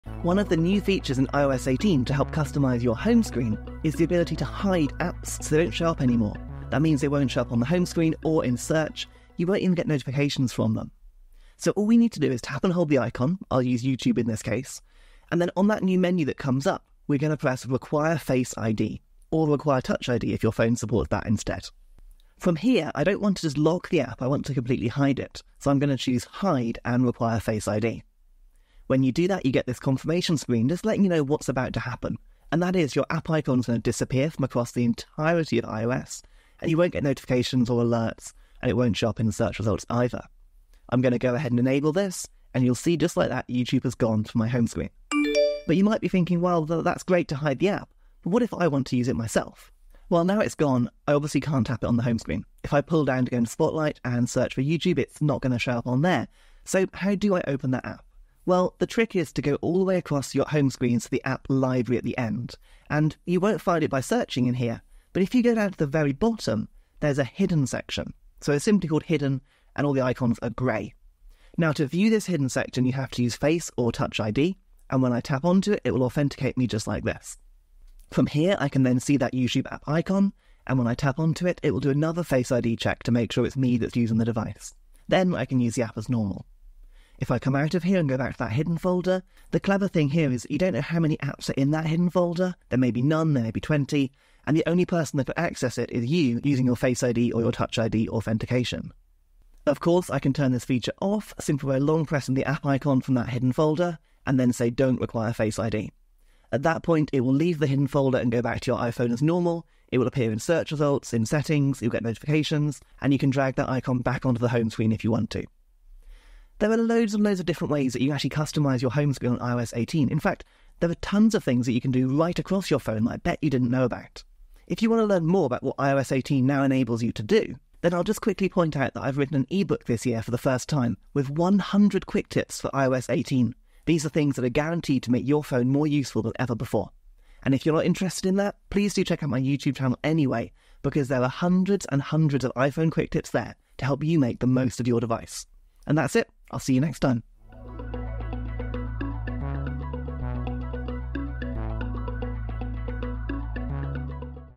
How to Hide Apps on iOS 18
In this video, I’ll show you how to use iOS 18’s new feature to hide apps from your home screen and search. Learn how to enable Face ID or Touch ID to hide and access your apps securely in the hidden section of the App Library. This step-by-step guide ensures your apps remain private, and you’ll know how to unhide them whenever needed.